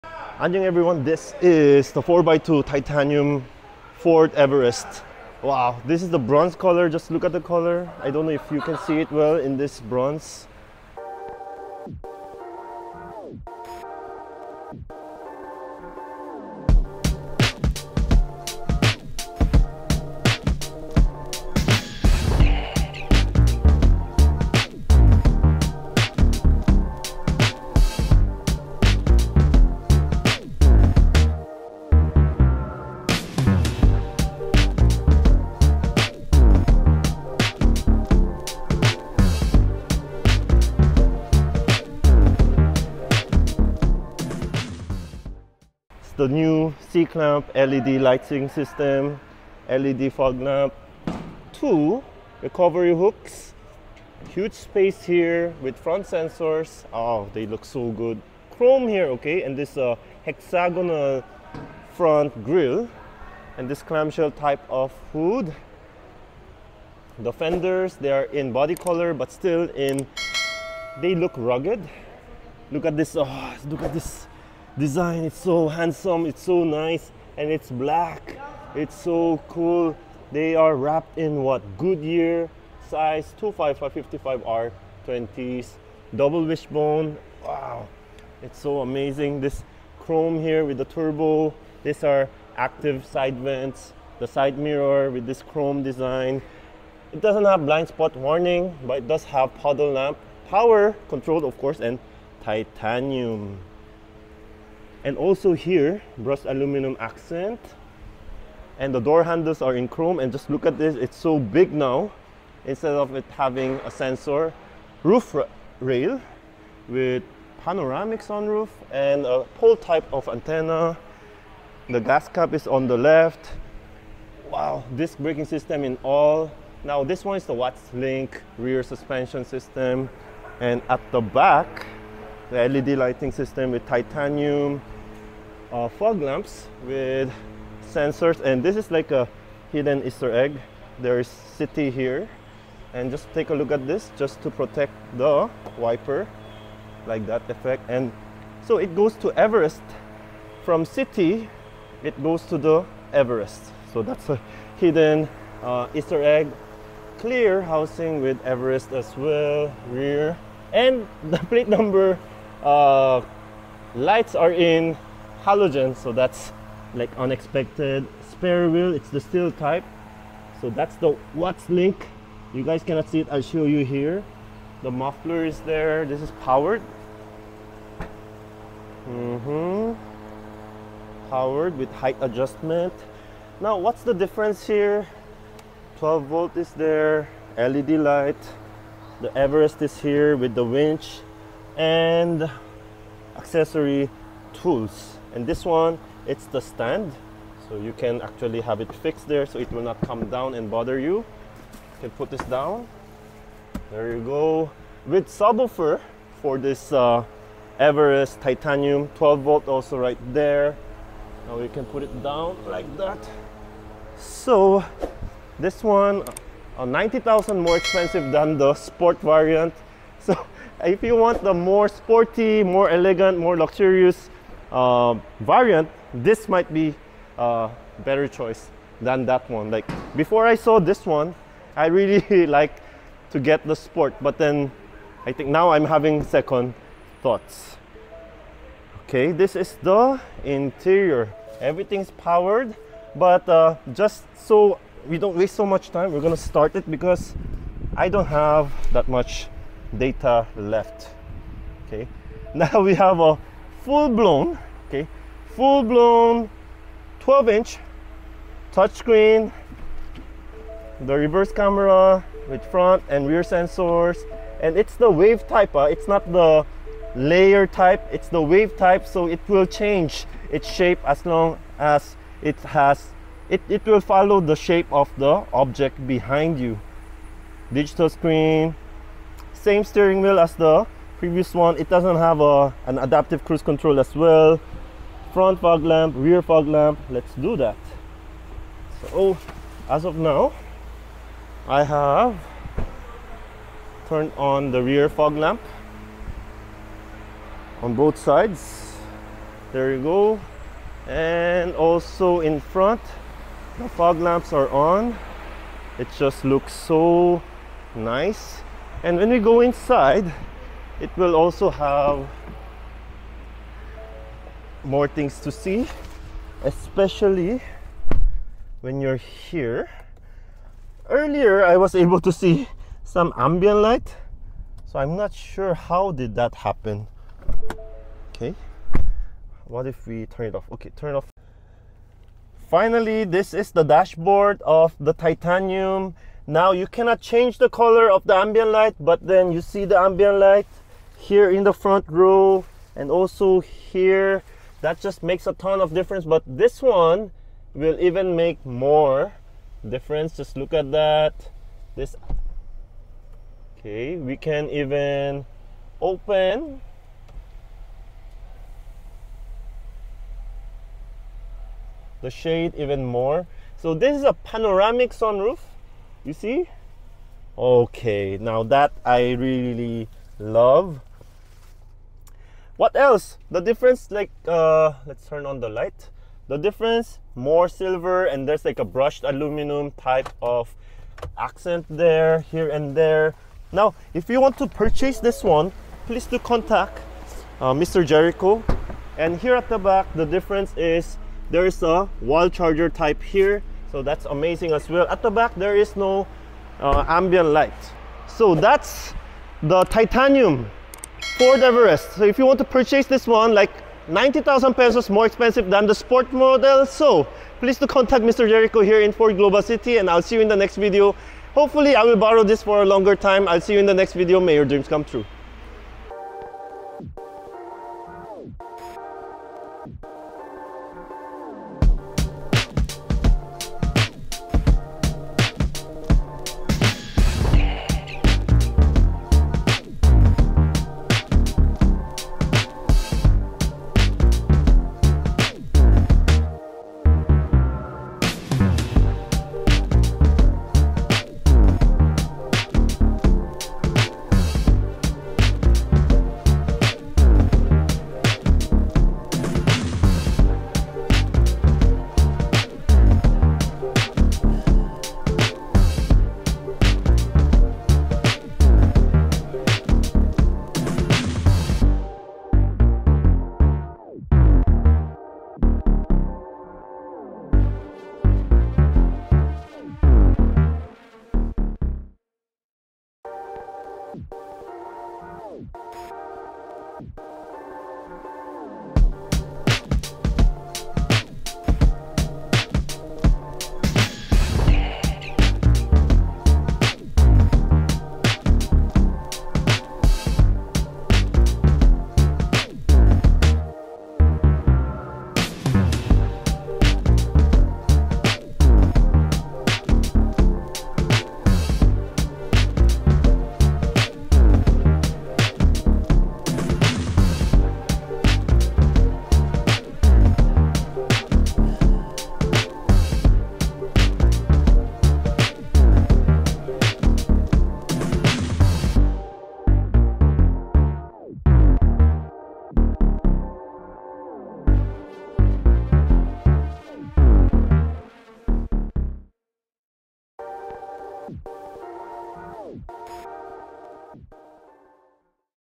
2023 Ford Everest Titanium Plus 4x2 Turbo 6 Speed Car Review
2023 Ford Everest Titanium Plus 4x2 Turbo 6 Speed Car Review

Ford Global City (Pls Mention SoJooCars)
Look for: Mr. Jericho Janolo (22 years FORD Expert)
Use Code: FordAHsojoo (freebies)